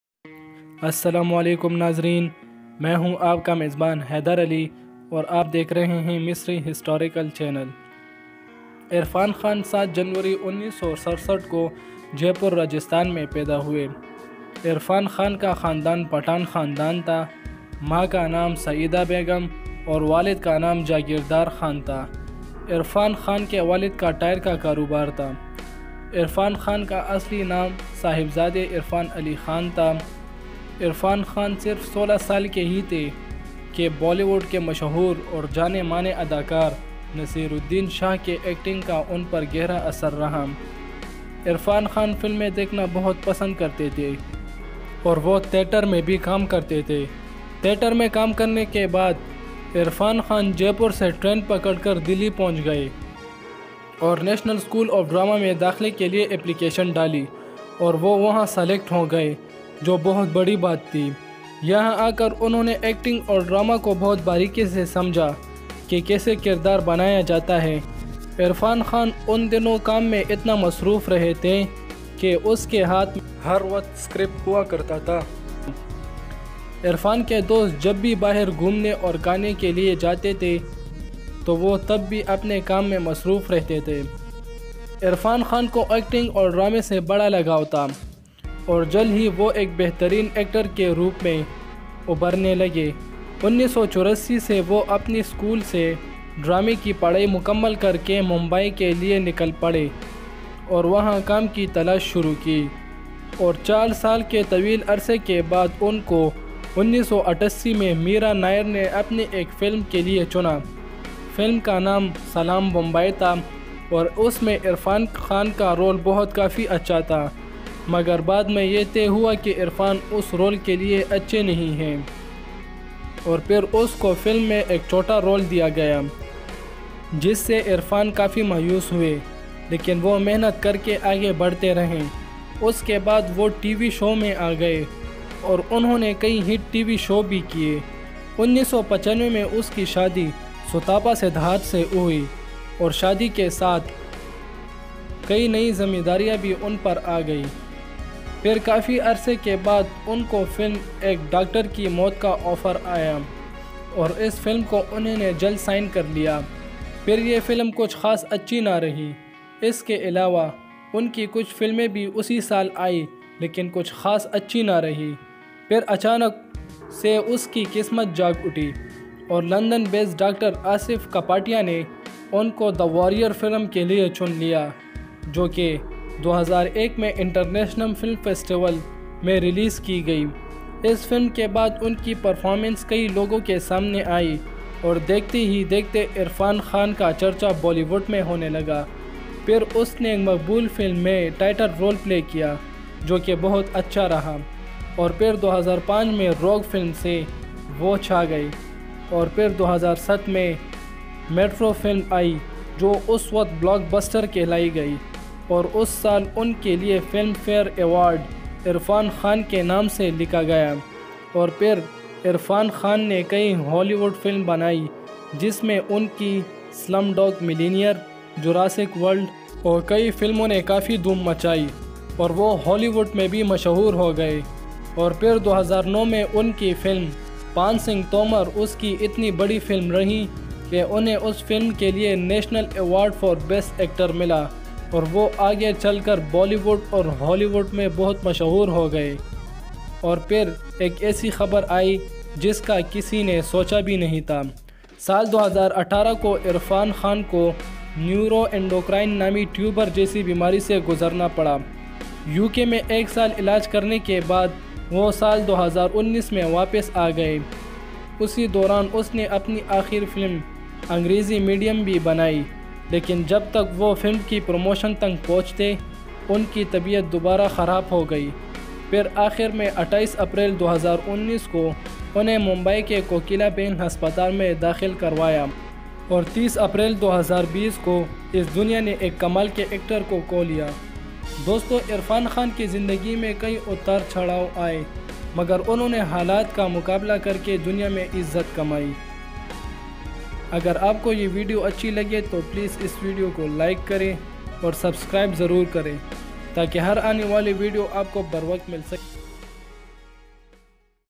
Irrfan khan Bollywood legendary actor biography
In this video belong to the Irfan Khan which was  legend actor.Irfan Khan born on 1967. He is very amazing actor ever.He born in rajistan and his family is pathan.After all he gone to national school of drama and graduated.Irfan Khan such a unique talent his first film is salaam Bombay but not gone well after a long struggle he get more film and success both Hollywood and Bollywood.